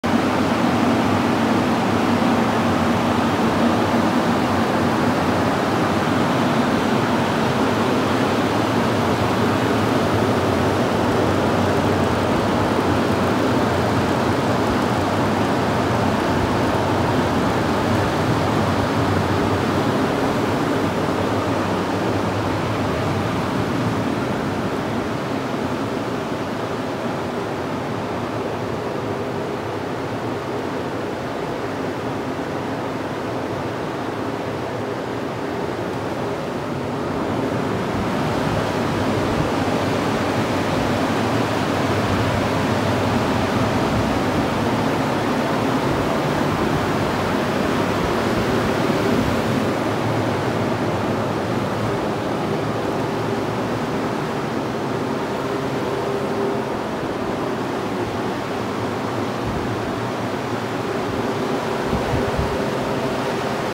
Exterminate bed bugs with a heat treatment
See a heat treatment in progress with our walkthrough video.